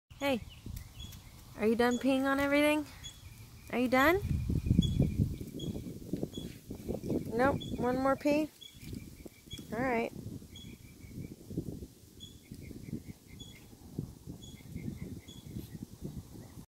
Miniature Pinscher marking his territory.
Tj out in the front.